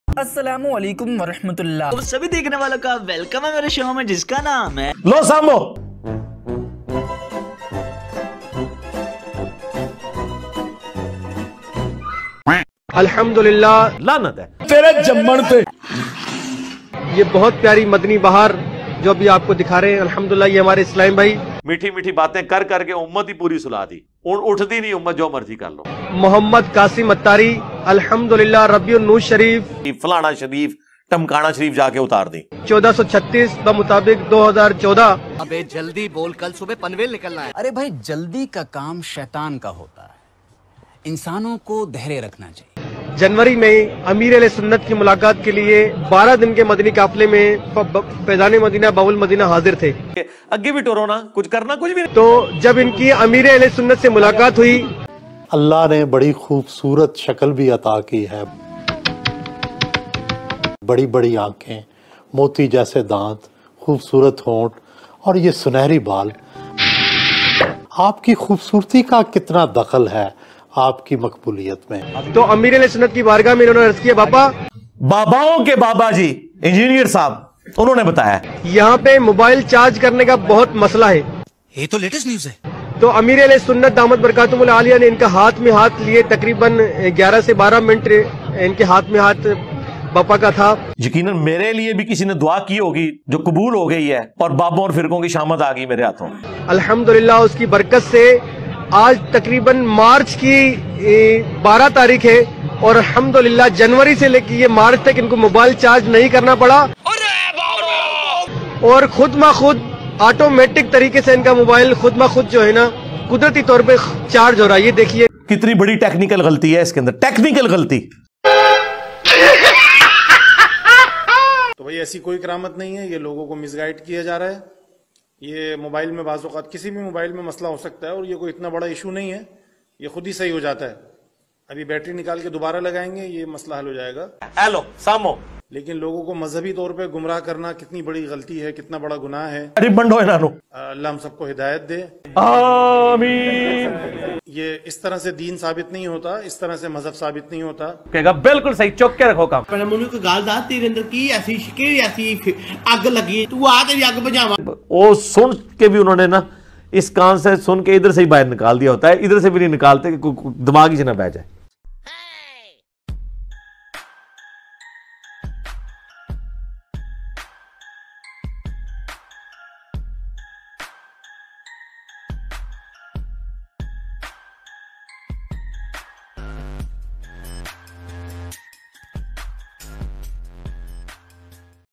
😡New Fake Karamat Exposed Mobile Phone Charge of Ilyas Qadri | Lo Sambho | Engr Muhammad Ali Mirza
In this video, we take a deep dive into the false stories about Mola Ali علیہ السلام that have been circulating online, and respond to Shehenshah Naqvi's claims point-by-point. We expose the lies and fabrications that have been spread about Mola Ali علیہ السلام, and provide a comprehensive and evidence-based response to each of Naqvi's claims. If you're interested in learning the truth about Mola Ali علیہ السلام and want to separate fact from fiction, this video is a must-watch.